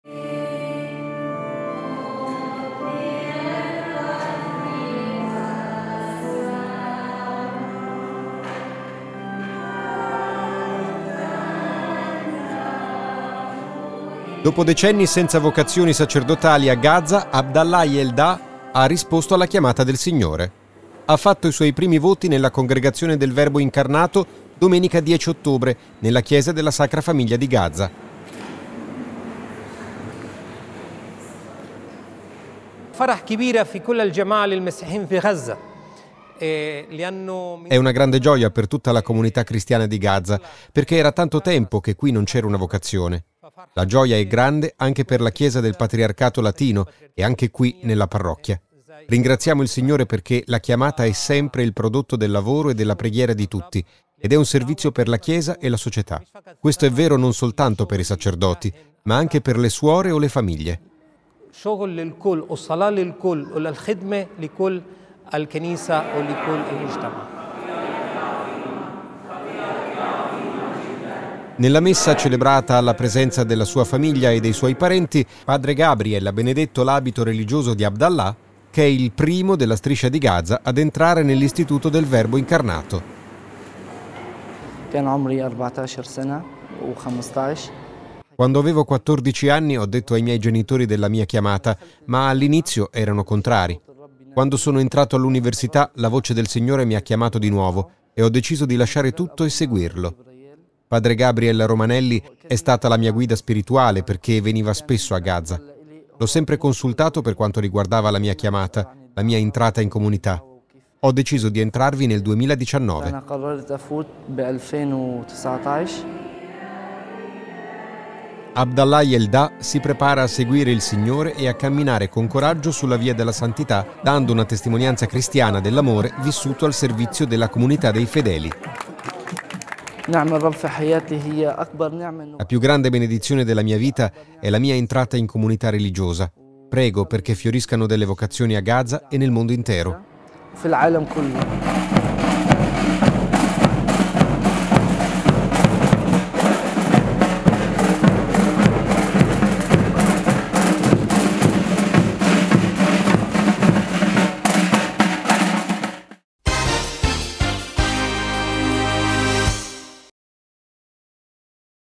A Gaza, dopo decenni, nasce una nuova vocazione
Dopo decenni senza nessuna vocazione, grande è la gioia della comunità cristiana di Gaza per il nuovo religioso, Abdallah Jeldah, che ha professato i primi voti. La messa è stata celebrata da padre Gabriel Romanelli, parroco della parrocchia latina di Gaza, nella Chiesa del Patriarcato Latino della città.